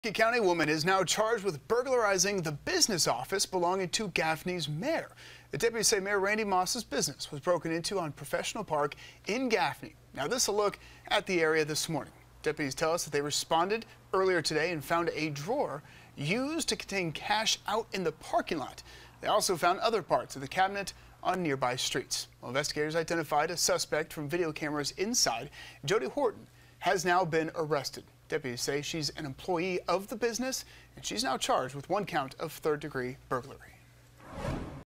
Burglary at Gaffney mayor's business office
The suspect was identified through security camera footage.